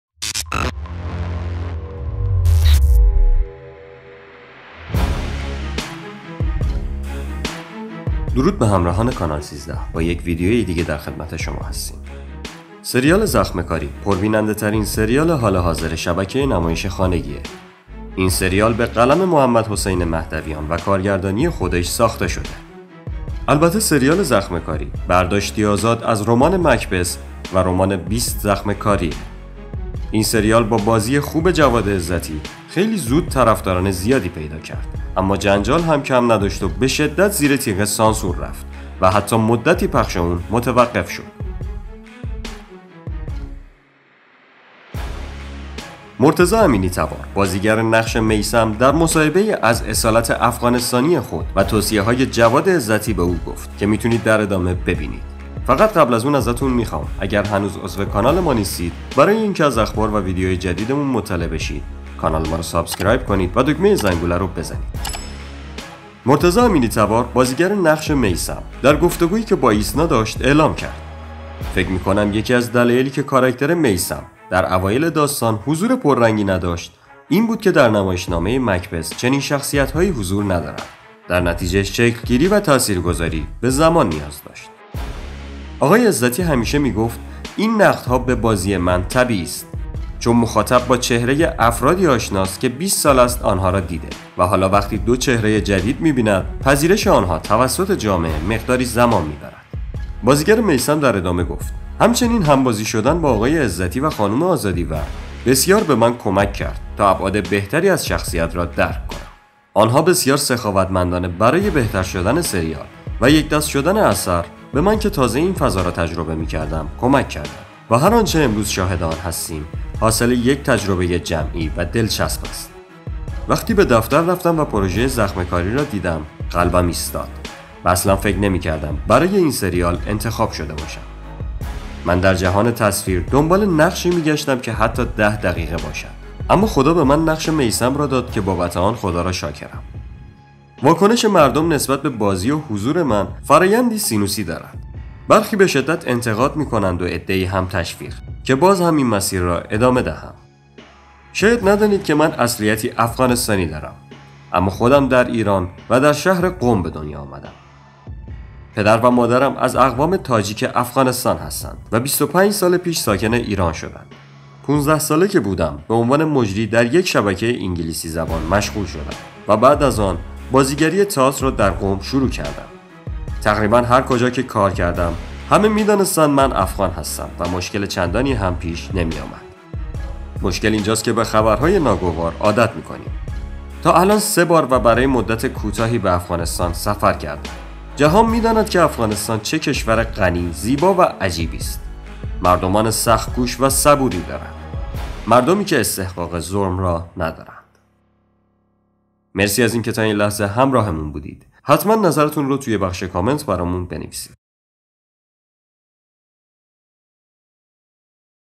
بازیگر سریال زخم کاری: اصالت افغانی دارم و توصیه هایی که جواد عزتی به میثم زخم کاری کرد
سریال زخم کاری پر بیننده ترین سریال حال حاضر شبکه نمایش خانگی می باشد که از پلتفرم فیلیمو پخش می شود . سریال زخم کاری به قلم محمدحسین مهدویان و کارگردانی خودش ساخته شده است . سریال زخم کاری براساس برداشتی ازاد از رمان مکبث و بیست زخم کاری می باشد . این سریال با بازی خود جواد عزتی خیلی زود طرفداران زیادی پیدا کرد اما جنجال هم کم نداشت . سریال زخم کاری به شدت زیر تیغ سانسور رفت این سریال از سانسور در امان نبود و حتی مدتی پخش آن متوقف شد .
مرتضی امینی تبار بازیگر نقش میثم در مصاحبه ای از اصالت افغانستانی خود و توصیه های جواد عزتی به او گفت که میتونید در این ویدیو ببینید